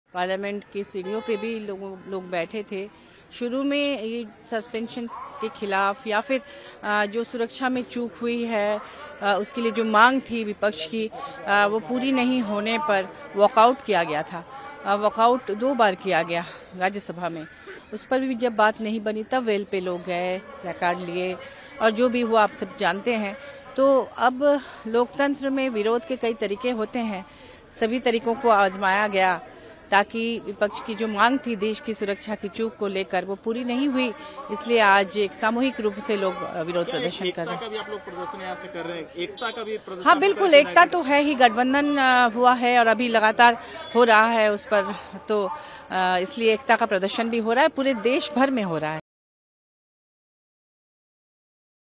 महुआ ने विपक्षी सांसदों के निलंबन के विरुद्ध इंडिया ब्लॉक के प्रदर्शन पर प्रतिक्रिया दी
झारखंड मुक्ति मोर्चा की सांसद महुआ माजी ने एएनआई से बात करते हुए विपक्षी सांसदों के सामूहिक निलंबन के खिलाफ इंडिया ब्लॉक के विरोध पर टिप्पणी की। उन्होंने कहा कि वे एकजुट होकर विरोध कर रहे हैं.
उन्होंने कहा, ''विरोध करने के कई तरीके हैं. इसलिए यहां हम एकजुट होकर विरोध कर रहे हैं।”